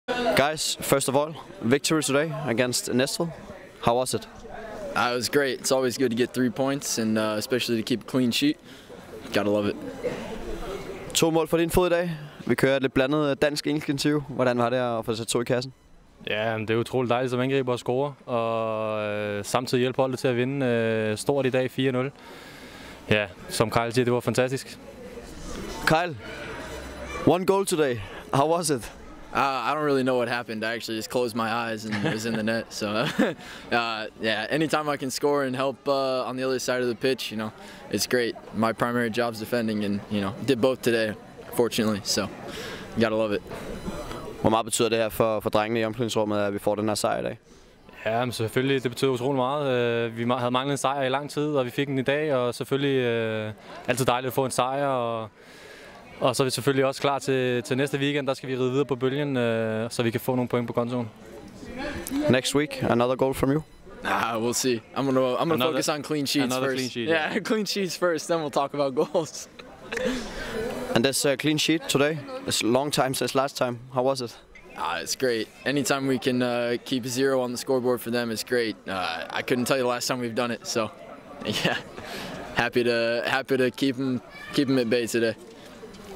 FCRTV: Lindberg & McLagan: Gotta love it
Søndagens sejr satte smil på læberne. Smilet var dog ekstra bredt på Kristian Lindberg & Kyle McLagan, der tilsammen stod for tre af kampens fire scoringer. Hør hvad drengene tænkte kort efter slutfløjt, i dette dansk/engelske-interview.